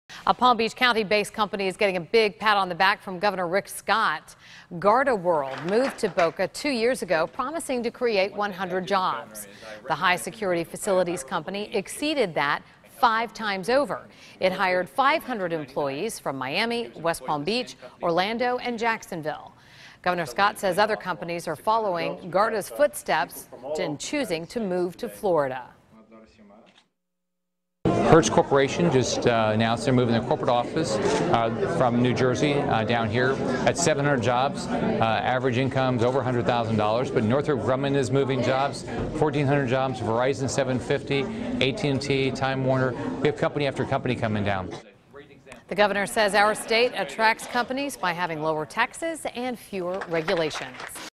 GardaWorld
Gets pat on the back from Governor Rick Scott